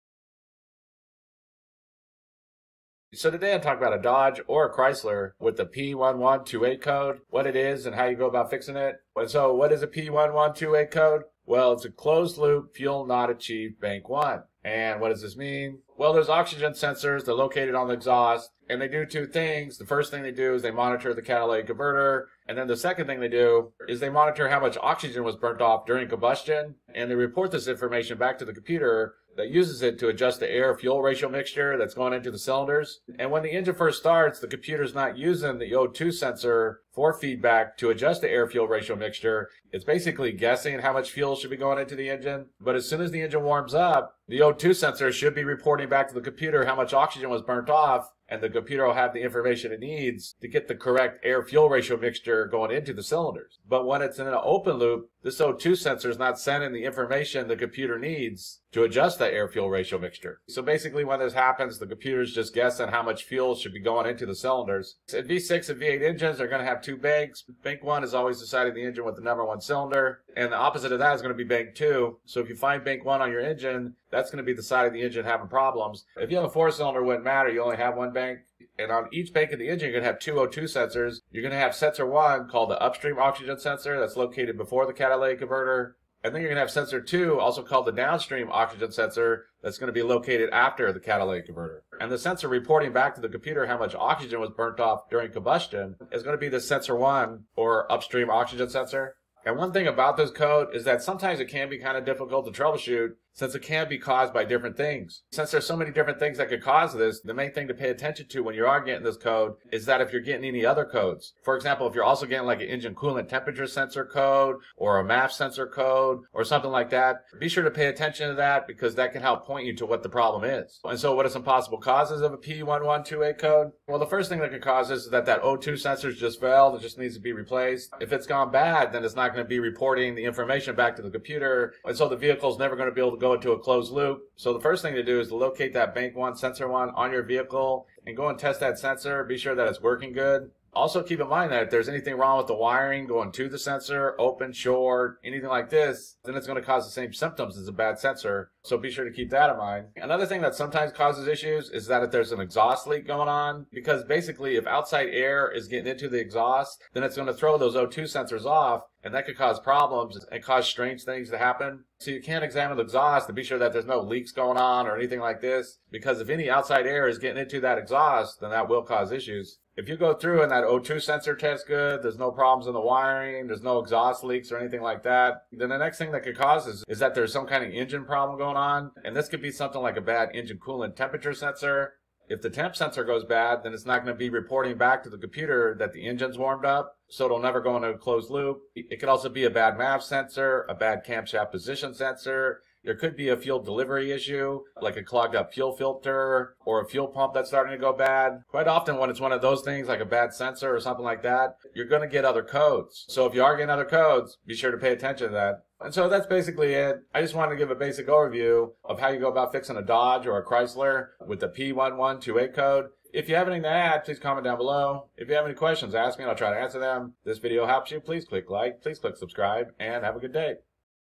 Causes and Fixes Dodge Chrysler P1128 Code: Closed Loop Fuel Not Achieved Bank 1
How to fix a Dodge Chrysler P1128 engine code: Closed Loop Fuel Not Achieved Bank 1
Faulty Air Fuel Ratio Sensor-1 Bank-1
Wiring - Open Short - Blown Fuse
Exhaust Leak
Engine Issue (Engine Coolant Sensor - MAF Sensor - Cam Position Sensor - Fuel Delivery)